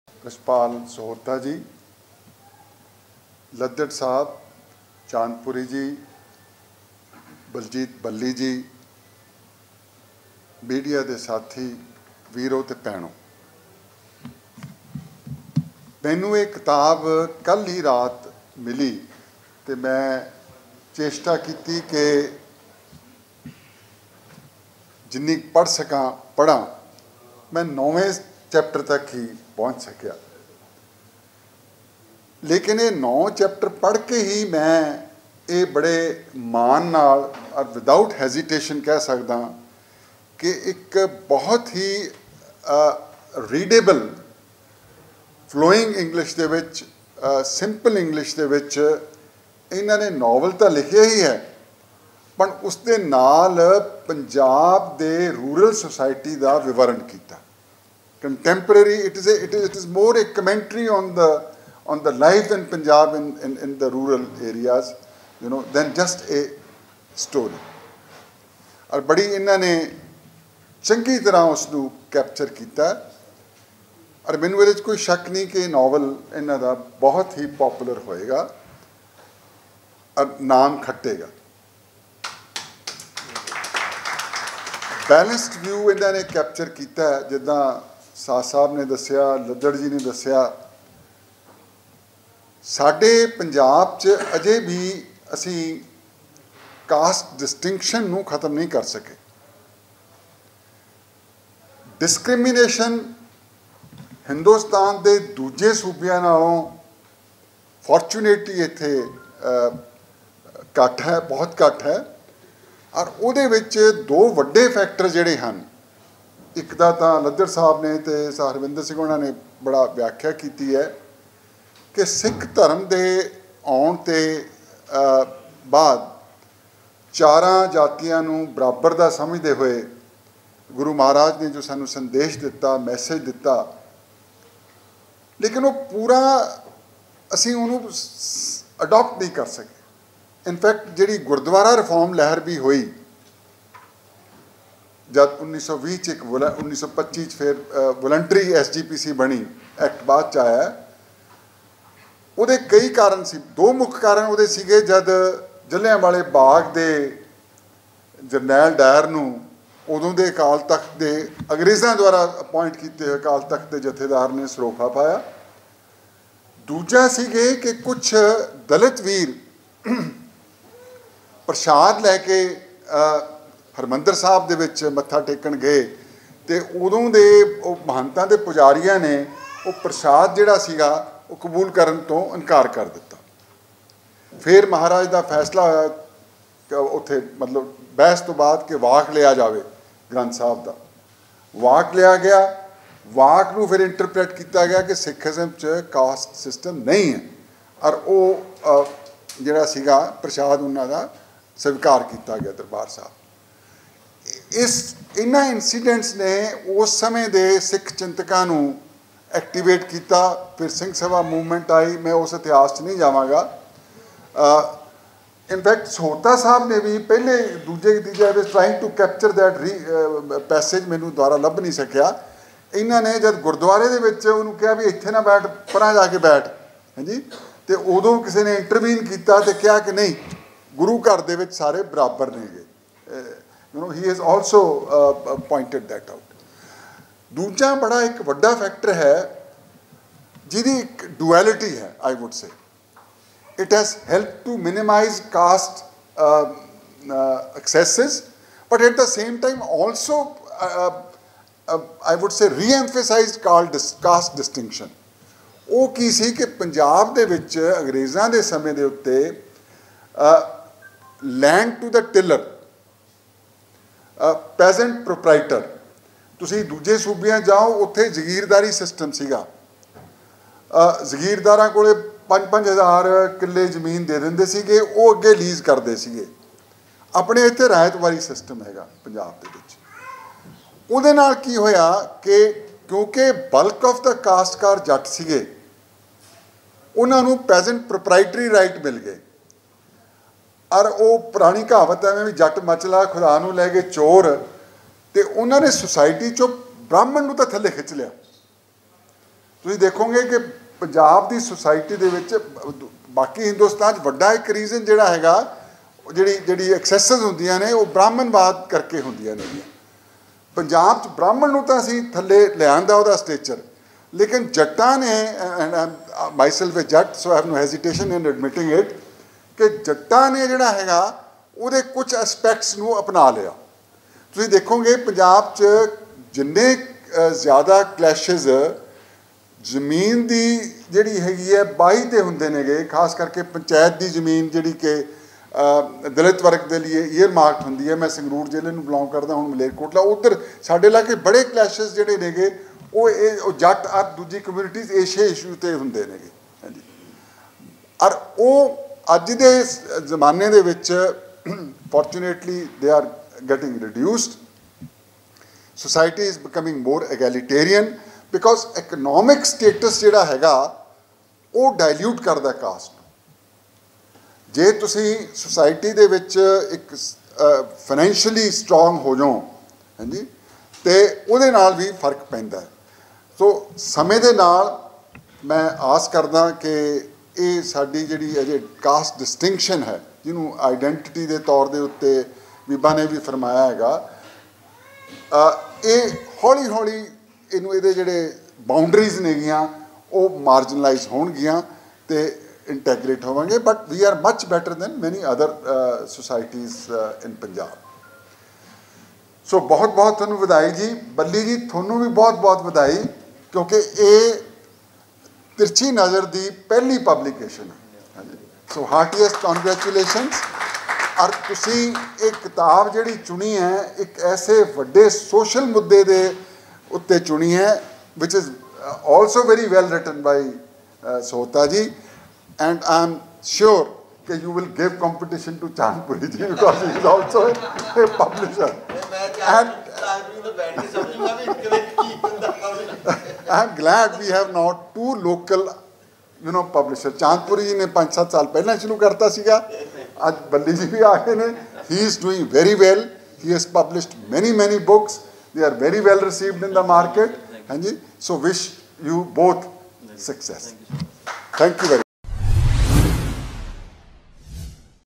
Ramesh Inder Singh on 'Chasing Dignity' - ਭਾਸ਼ਾ ਬੇਹੱਦ Simple l Novel 'ਚ Rural Punjab ਮੁਕੰਮਲ ਝਲਕ..
Ramesh Inder Singh on 'Chasing Dignity' - ਬੇਹੱਦ Simple ਭਾਸ਼ਾ ਚ ਲਿਖੀ ਇਹ Novel ਚ Rural System ਦੀ ਝਲਕ ਮਿਲਦੀ ਹੈ